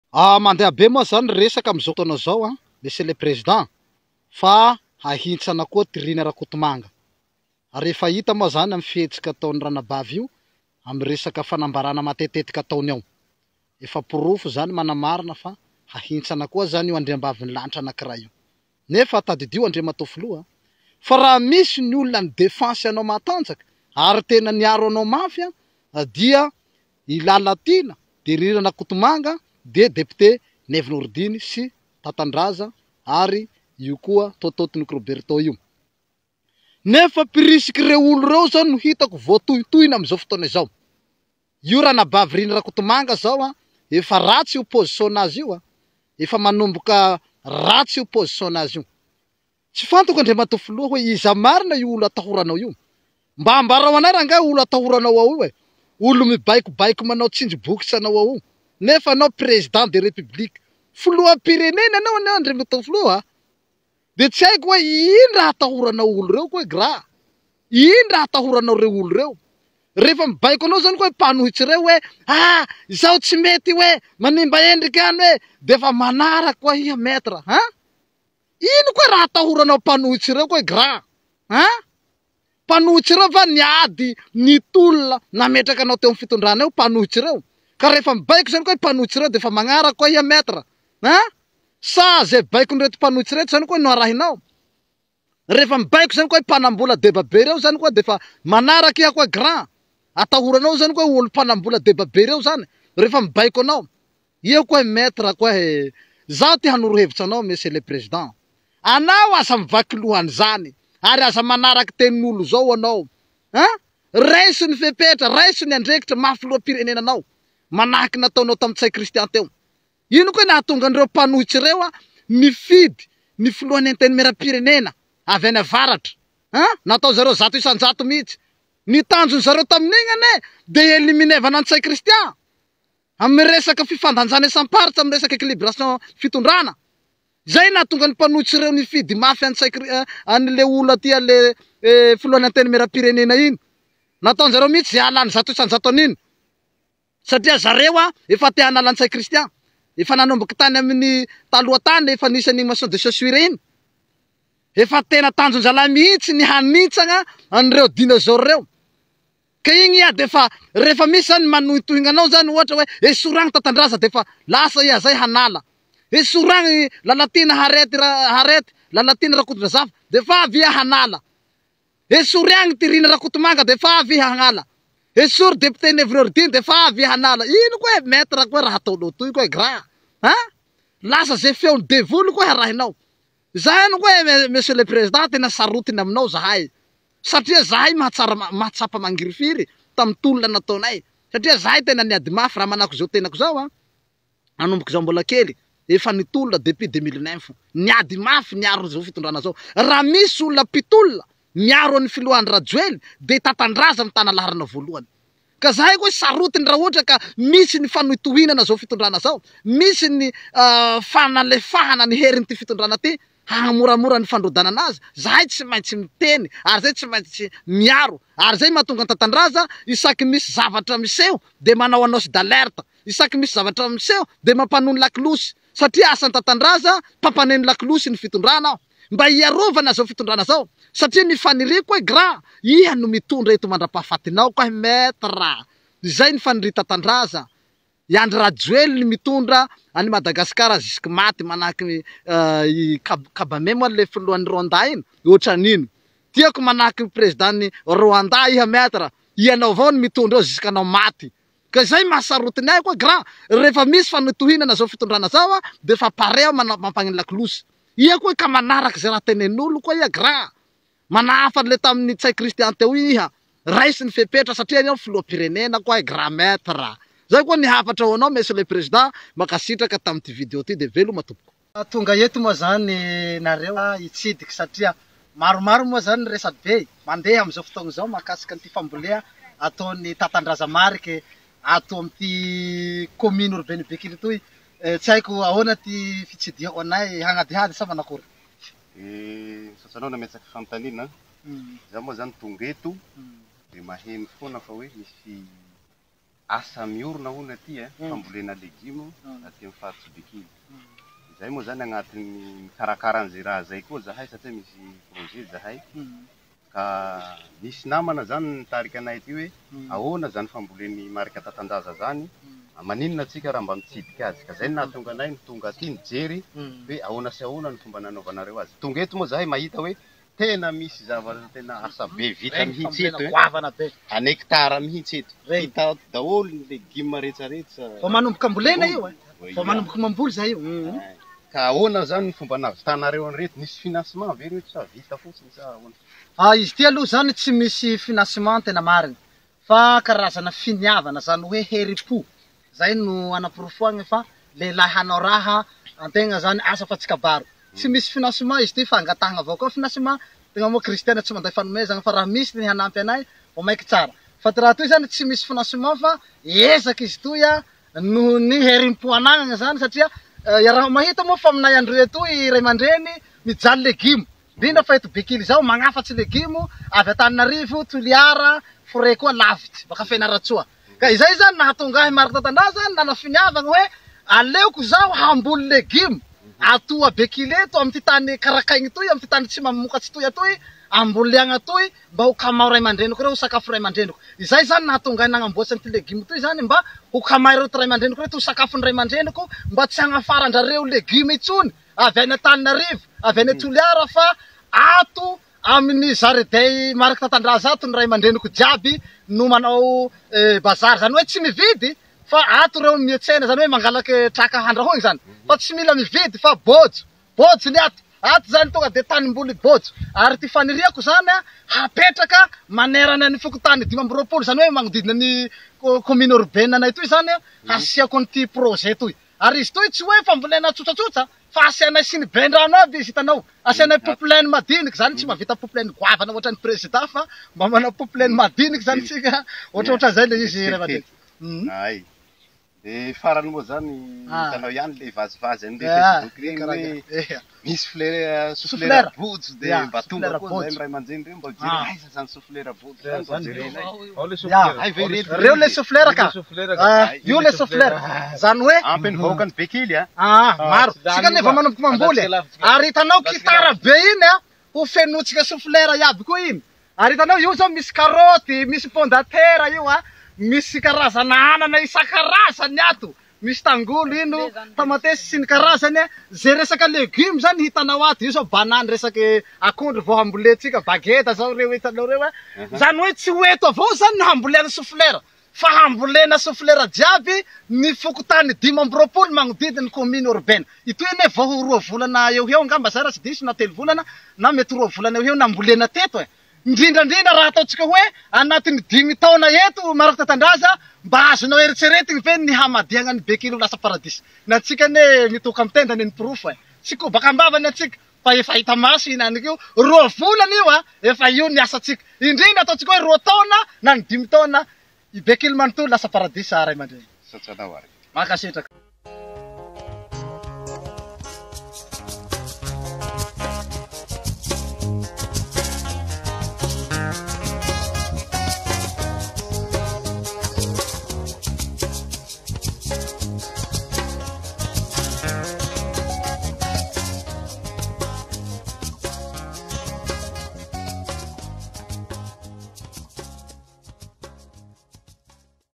Marc Tatandraza - HAHINTSANA IHANY KOA i Rina RAKOTOMANGA ...
MISY OLONA MPANAM-BOLA ATAHORANY NY FILOHAM-PIRENENA TSIKA , IZA ARY IO ??? ...

AZA ADINO MI ABONNÉ NY CHAÎNE ✅